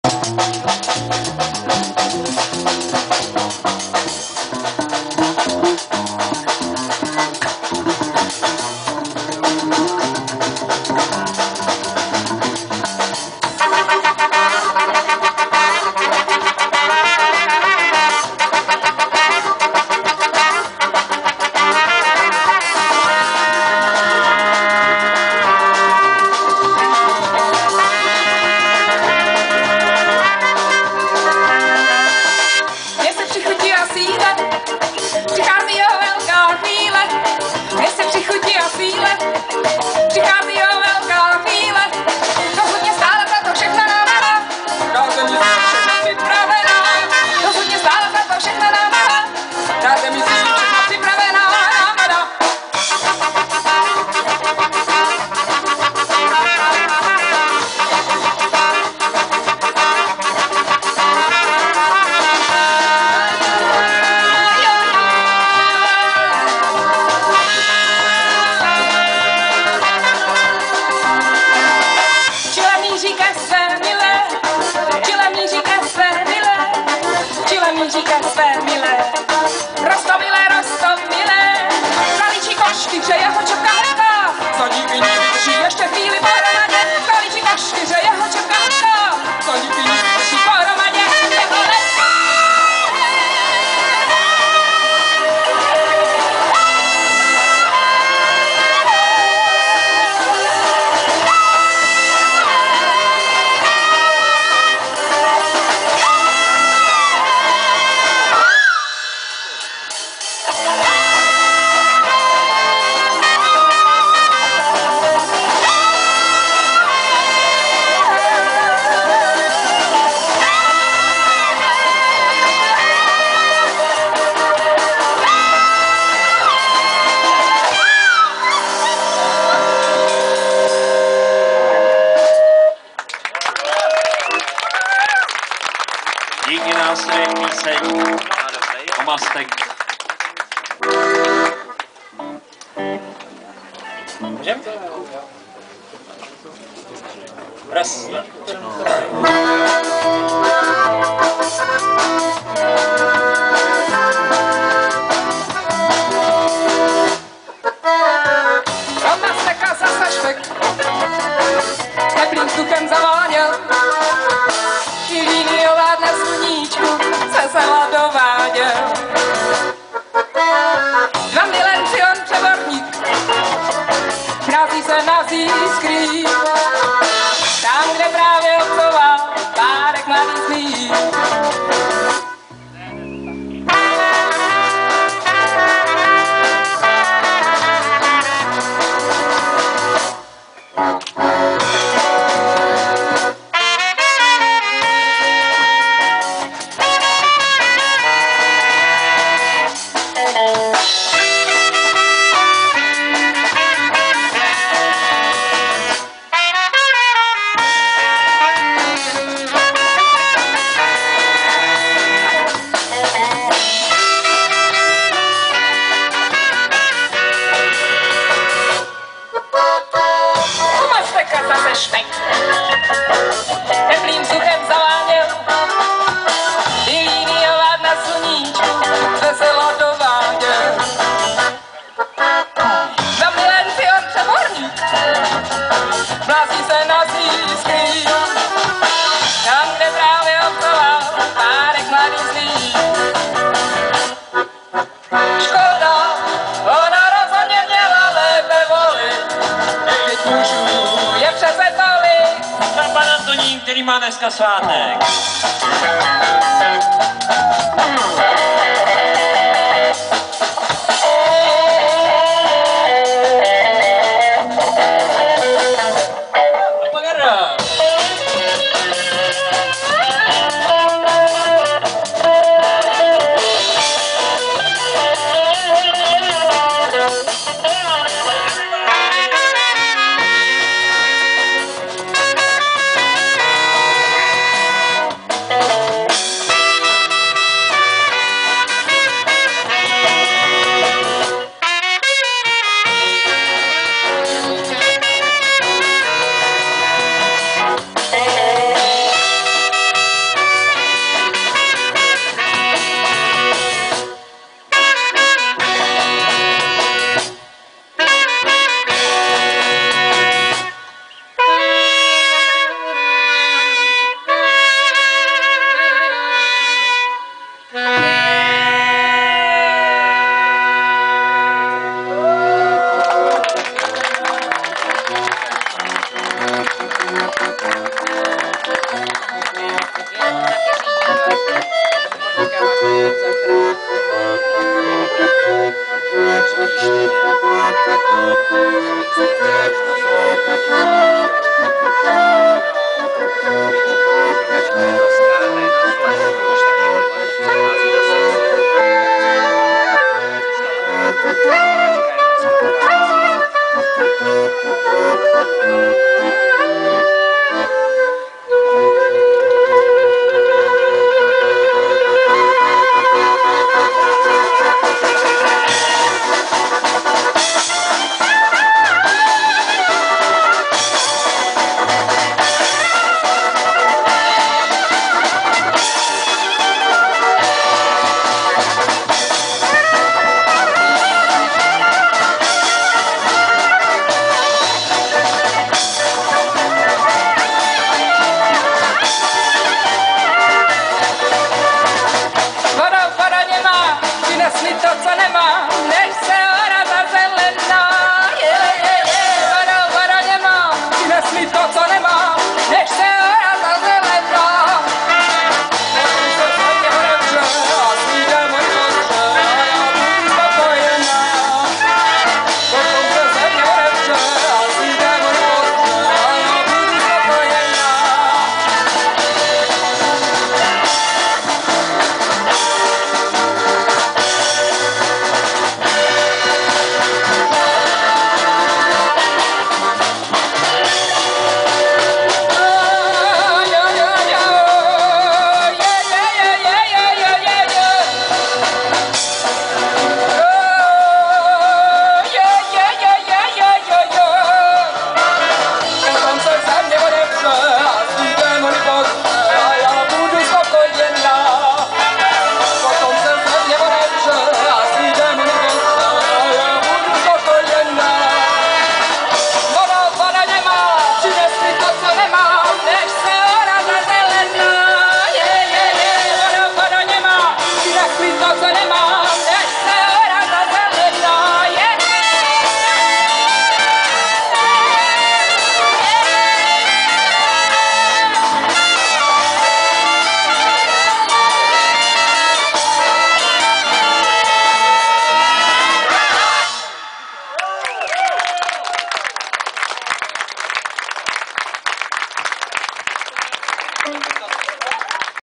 Neočekávaný dýchánek - Kladenské dvorky 2010 - pokračování 1
Amatérské video bez úprav z koncertu Neočekávaného dýchánku pod širým nebem v rámci akce Kladenské dvorky 2010.